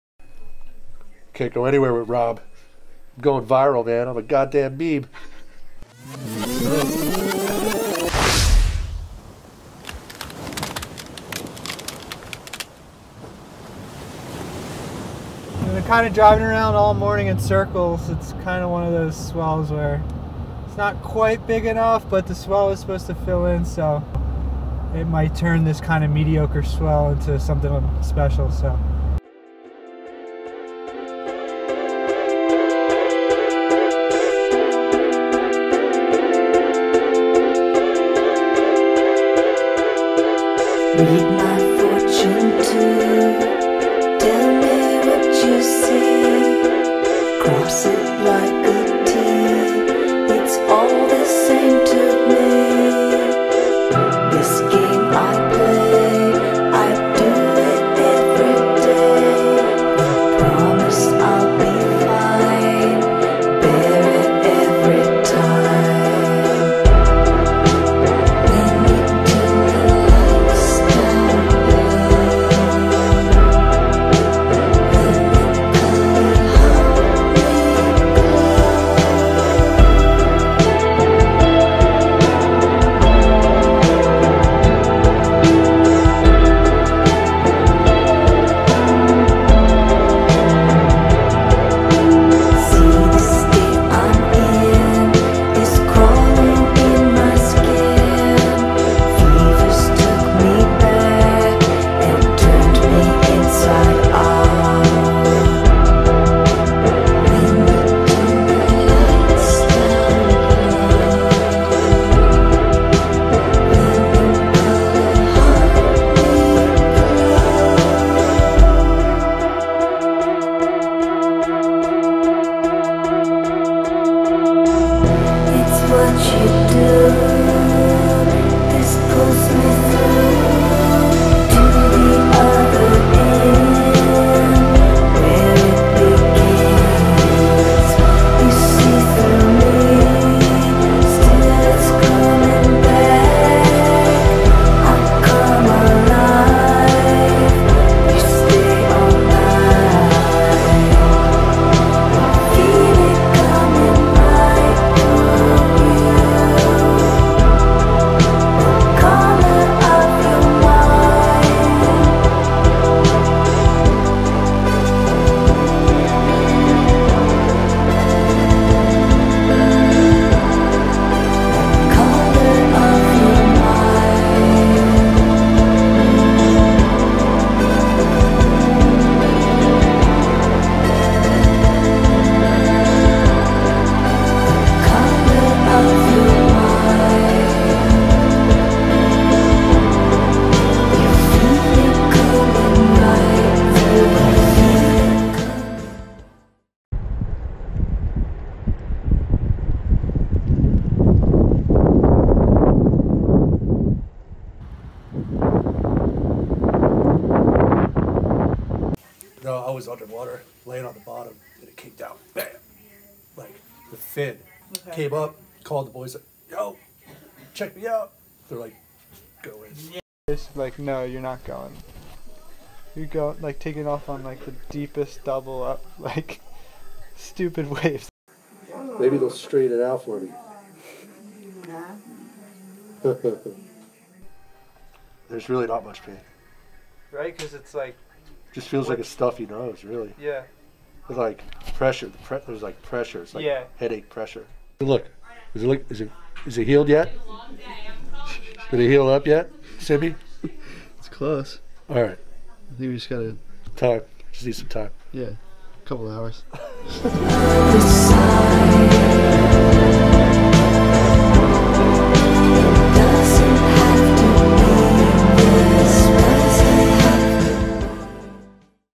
Numb Skulls | Get Barreled Till Your Nose Falls Off
What started as a classic winter session in New Jersey ended in the emergency room after Gesler took a fin straight to the nose and got one of the gnarliest gashes we've ever seen.

Film/Edit: Ryan Simalchik
Additional footage: Brian Bacich
Surfers: Rob Kelly, Sam Hammer, Will Skudin, Mike Gleason